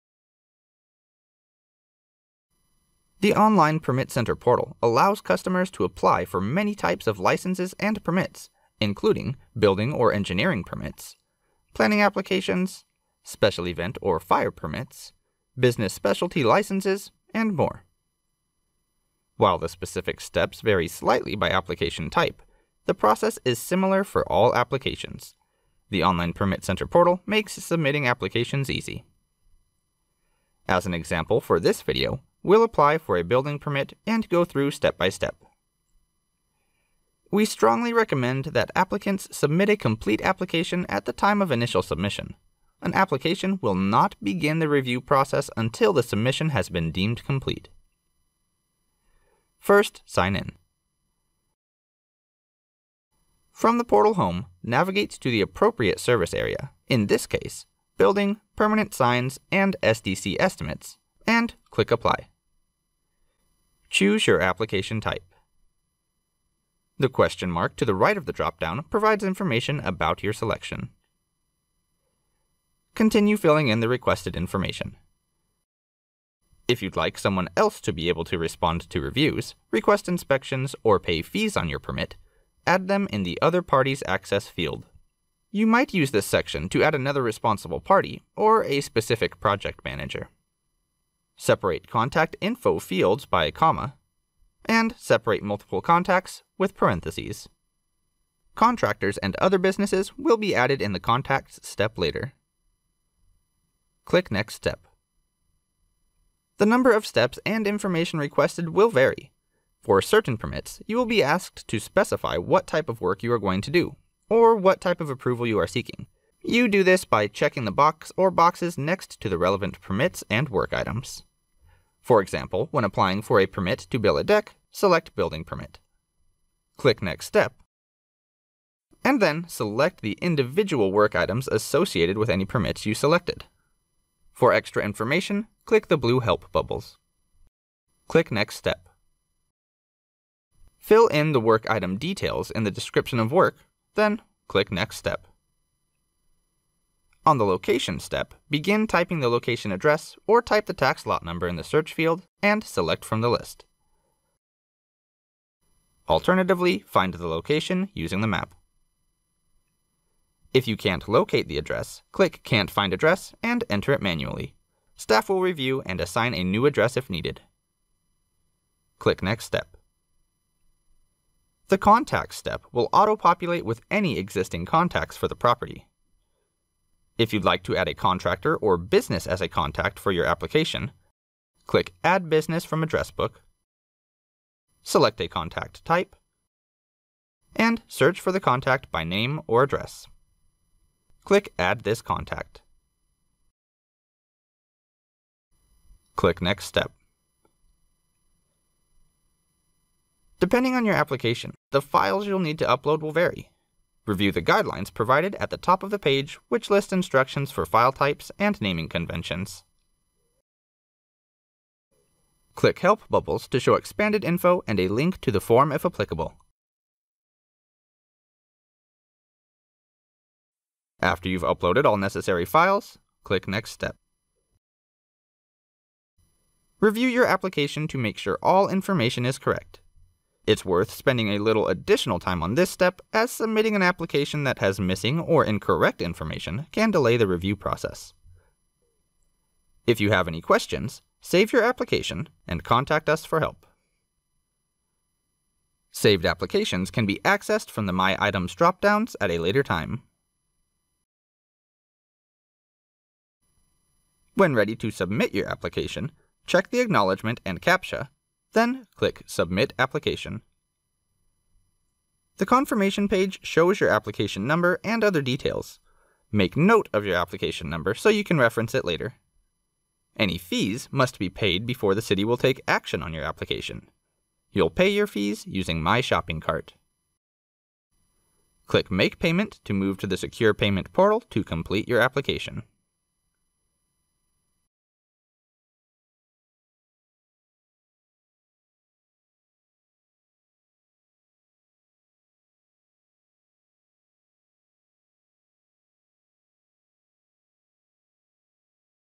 Submitting an Application
The Online Permit Center Portal allows customers to apply for many types of licenses and permits, including Building or Engineering permits, Planning applications, Special Event or Fire permits, Business Specialty Licenses and more.
While the specific steps vary slightly by application type, the process is similar for all applications. The Online Permit Center Portal makes submitting applications easy.
As an example for this video, we’ll apply for a Building Permit and go through step-by-step.